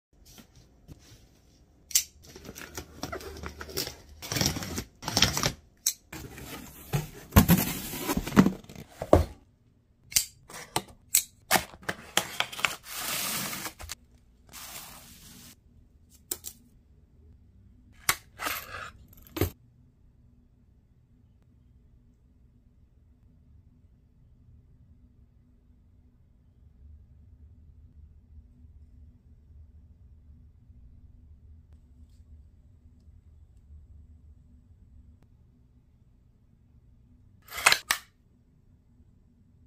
This is the MOST BADASS and COOLEST fixed blade to come out in a long time
Zero Tolerance ZT0006